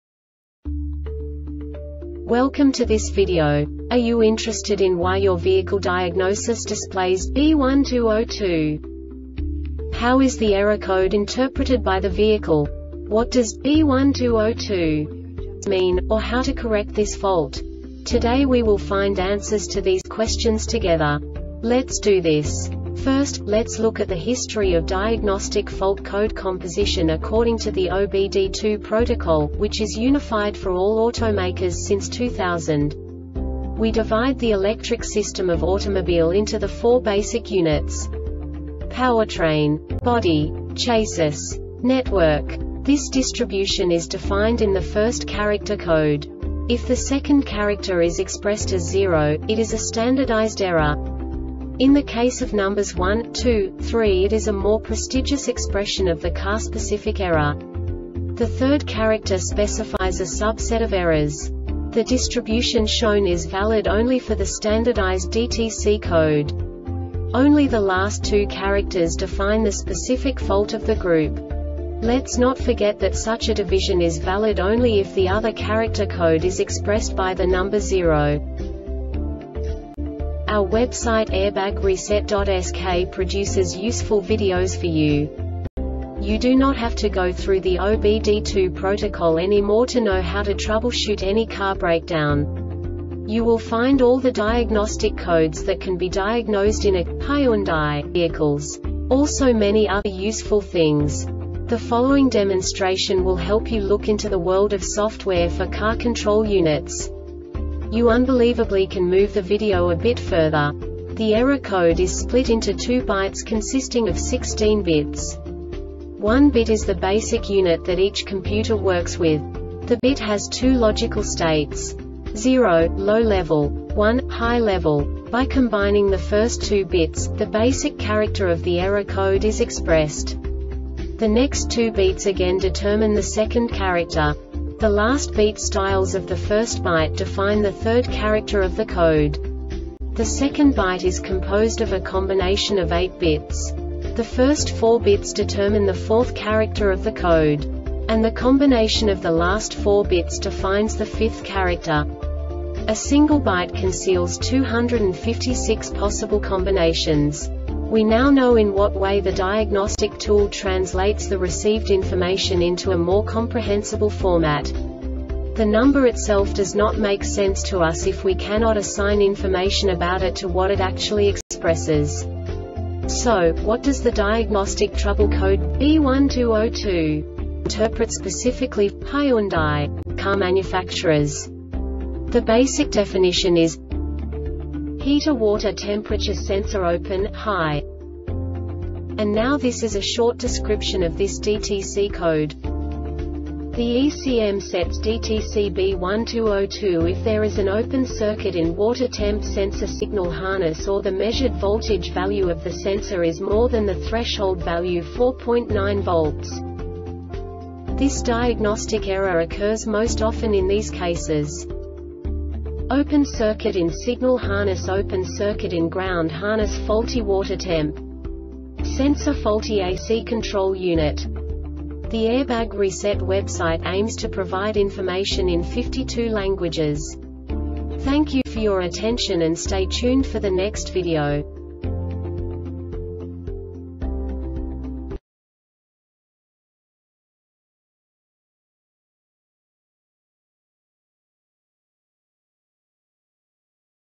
DTC Hyundai B1202 Short Explanation
Contents:
0:21 Basic DTC analysis according to OBD2 protocol standard.
1:48 Insight into programming
2:58 A diagnostically understandable description of the Diagnostic Trouble Code
3:05 Basic error definition

Make:
Hyundai
Code:
B1202
Definition:
Heater Water Temperature Sensor Open (High)
Description:
The ECM sets DTC B1202 if there is an open circuit in water temp sensor signal harness or the measured voltage value of the sensor is more than the threshold value(4.9V)
Cause:
Open Circuit in signal harness
Open Circuit in ground harness
Faulty water temp. Sensor
Faulty A or C Control Unit